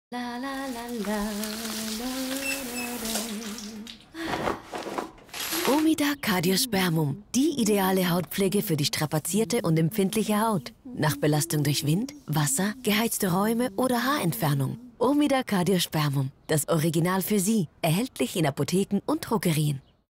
Omida Hautlotion Cardiospermum Halicar
TV commercial to promote the body lotion Cardiospermum Halicar of Omida.

Work:
Concept development, 2D animation

Client: Omida
Production: frame eleven

frame eleven
Creative Director: Marc Haas
Post Producer: Lilian Lüthi
2D Animation: Simon Piniel
Sound Design & Audio Post: Jingle Jungle